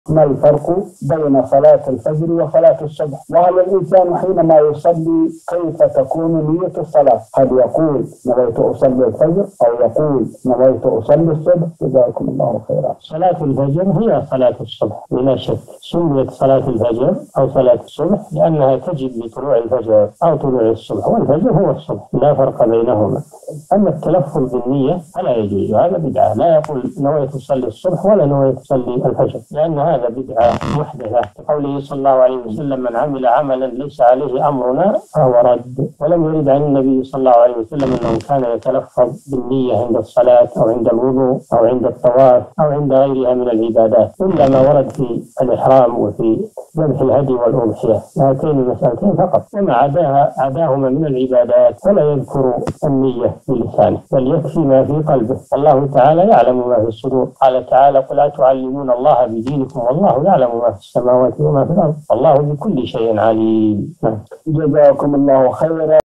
ما الفرق بين صلاة الفجر وصلاة الصبح وكيف تكون النية في الصلاة؟ الشيخ صالح بن فوزان الفوزان
نص السؤال:
ما الفرق بين صلاة الفجر وصلاة الصبح وهل الإنسان
عندما يصلي كيف تكون نية الصلاة هل يقول نويت
أصلي الفجر أو يقول نويت أصلي الصبح جزاكم الله خيراً؟
السيرة الذاتية :
للاشتراك في قناة الشيخ صالح الفوزان

رئيس الفريق
لمشروع كبار العلماء